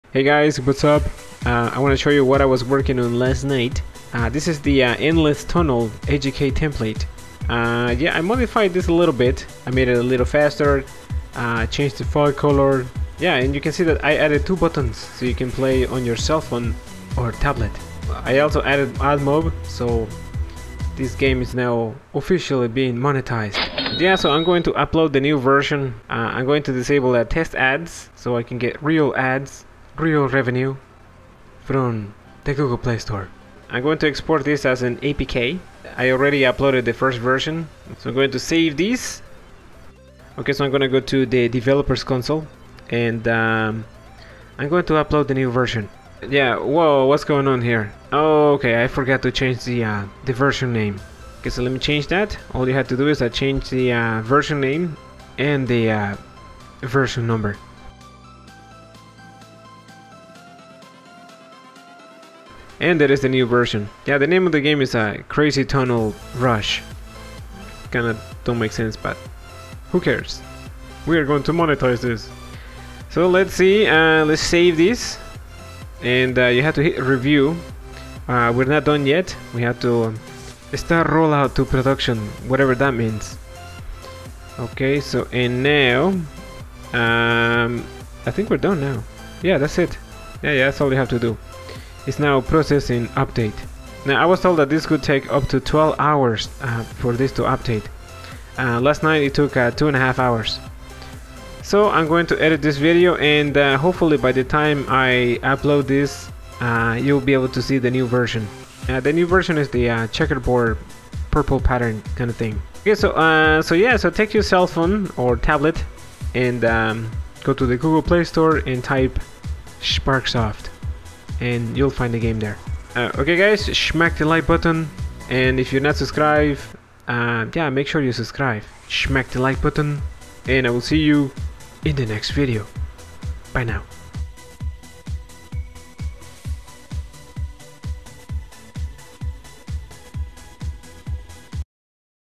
How I upload games to the GooglePlay store for Monetization - app game kit for developers
I'm not even kidding, making money with game development has never been so easy.

Search: Sparcksoft

If you found this into helpful consider tipping this channel.

App game kit has great templates for coders and non-coders. Monetization is as easy as saying pumpkin pie.

I made this 3D endless pipe rush using one of the AGK2 templates found in the app game kit. I'm using game pack 1.

Think about it, you'll spend weeks developing and testing, app game kit saves a ton of time, you can take an already developed template and shape it into whatever you like.

The important thing here is being able to generate revenue through ads. Abmob is great because the revenue is combined with your Youtube monetization, google adsense and admob.